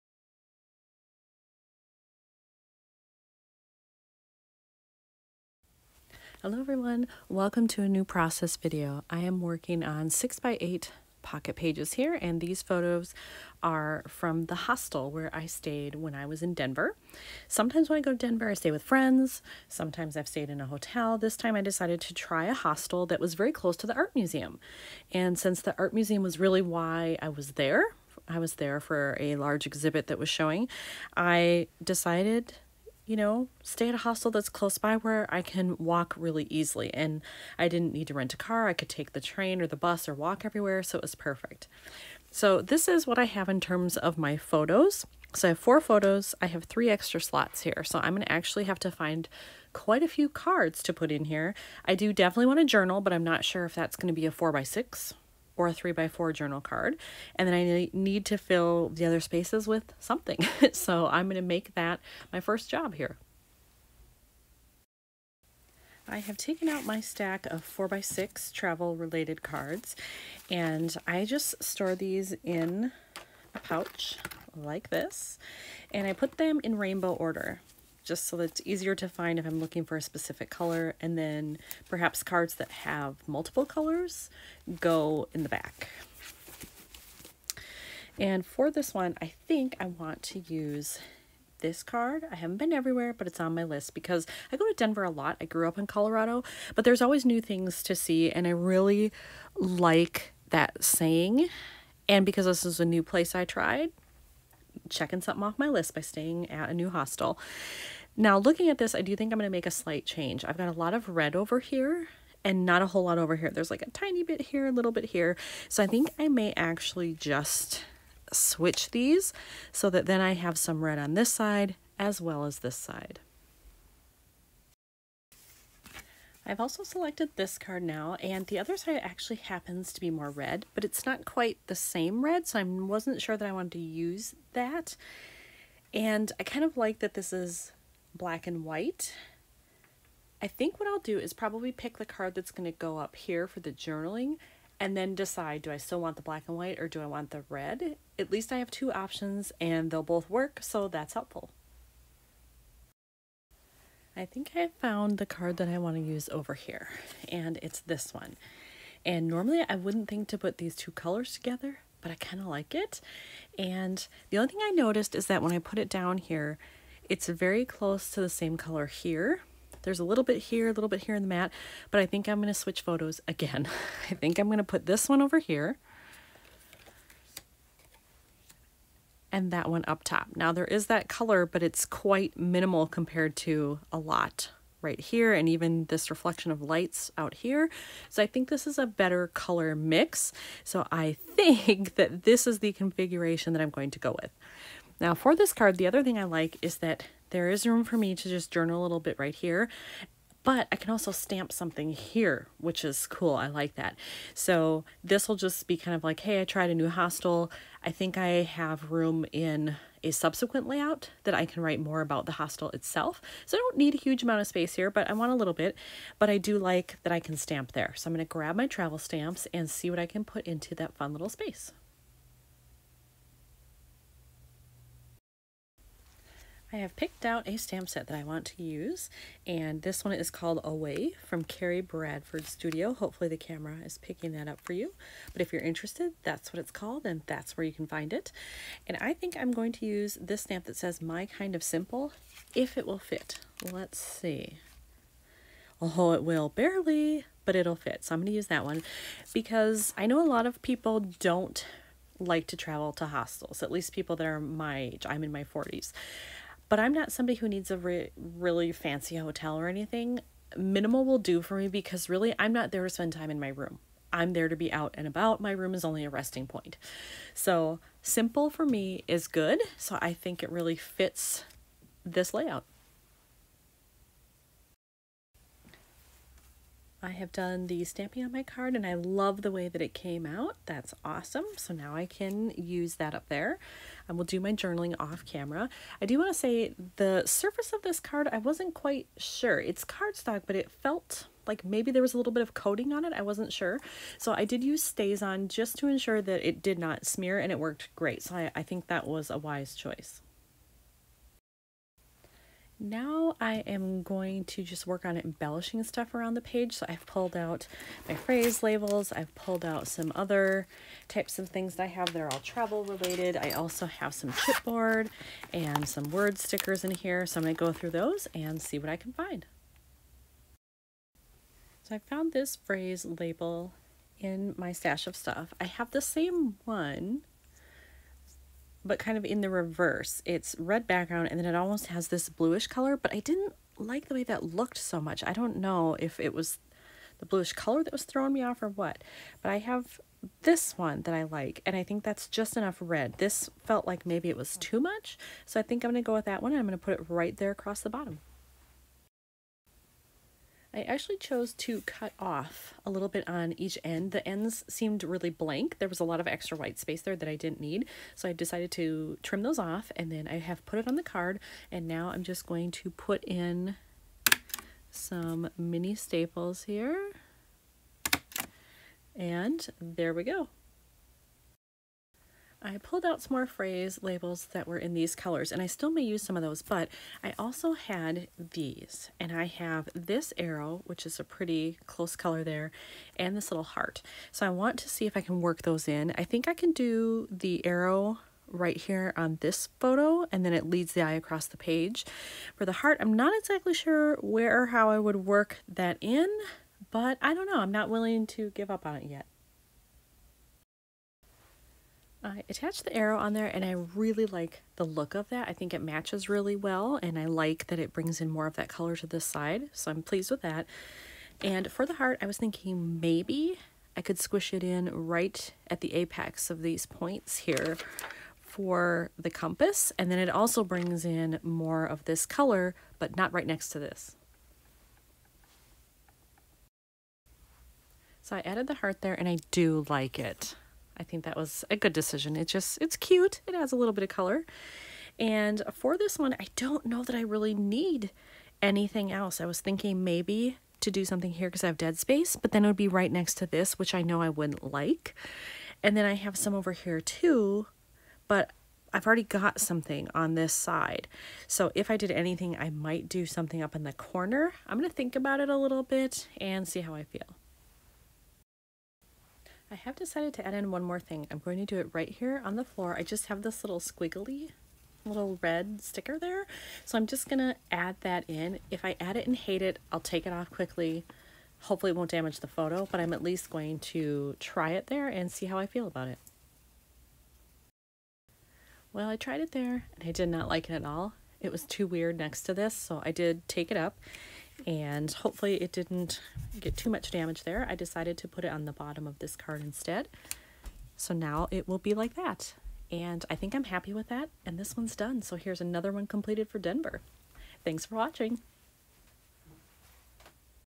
Denver: My Kind of Simple
This is a 6"x8" pocket page layout featuring photos from a fun hostel in Denver.  You'll see me use:  pocket cards, puffy stickers, phrase labels, stamping, and a regular sticker.